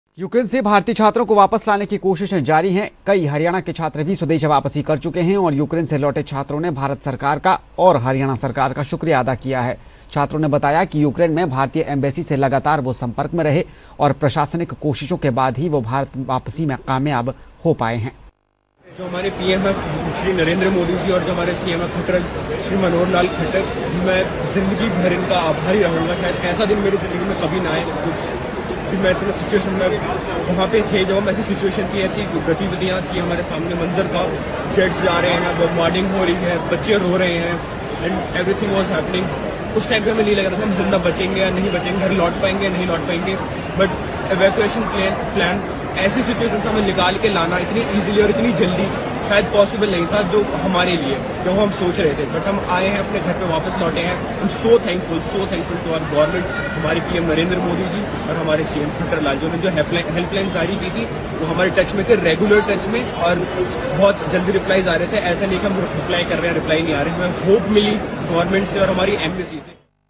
हरियाणा के कई छात्र भी यूक्रेन से भारत लौटे
हरियाणा के कई छात्र भी यूक्रेन से भारत लौटे। छात्रों ने भारत सरकार का शुक्रिया अदा किया। कहा–एंबेसी की मदद से हो पाई स्वदेश वापसी।